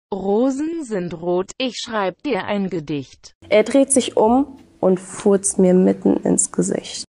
Er dreht sich um und furzt mir mitten ins Gesicht
Dieses Video bekommt genau 1762 Clicks nahui

Für all die Leute, die das Video nicht mehr auf tiktok finden können.